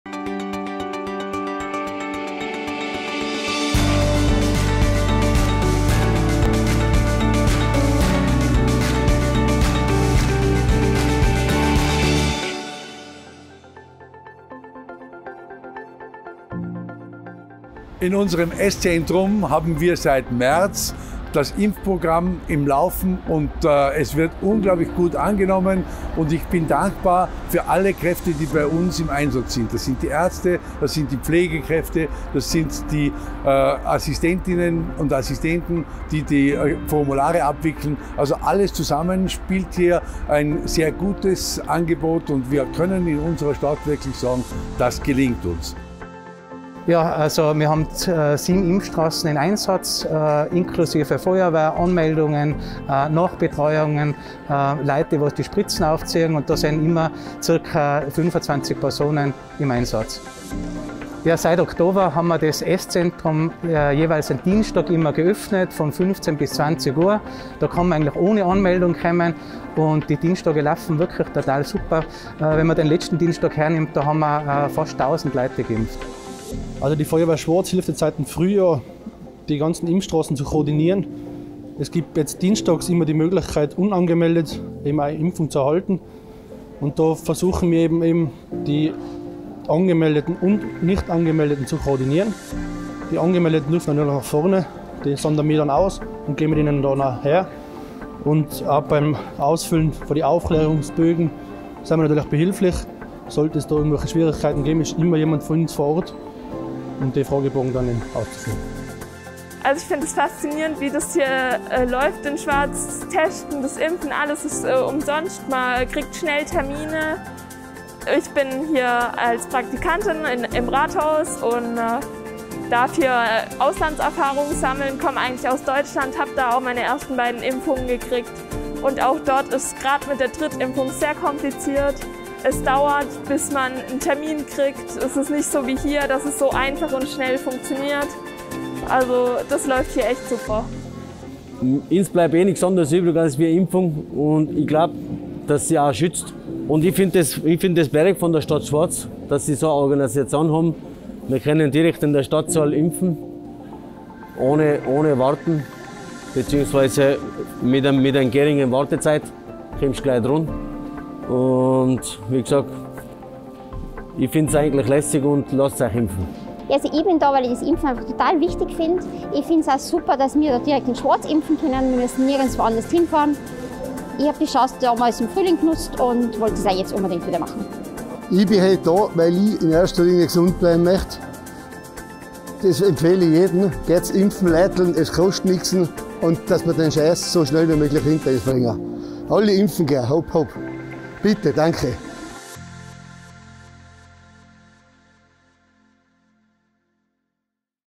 "Schwaz bewegt" - Impfzentrum Schwaz
Seit März 2020, mit Beginn der großen Impfaktion im Bezirk Schwaz, werden im Veranstaltungszentrum SZentrum Corona-Schutzimpfungen durchgeführt. In den letzten Monaten konnten die Verantwortlichen viel Erfahrung in der Organisation sammeln. Ständige Verbesserungen und Anpassungen gewährleisten einen reibungslosen Ablauf. Mindestens 25 Mitarbeiter:innen der Stadt Schwaz, Ärzt:innen, Pflegefachkräfte und Mitglieder der Freiwilligen Feuerwehr sind dort bei jedem Termin im Einsatz.
In der Reihe "Schwaz bewegt" konnten wir einen Blick hinter die Kulissen wagen und haben uns ein Bild gemacht, wie die Impfung im SZentrum abläuft.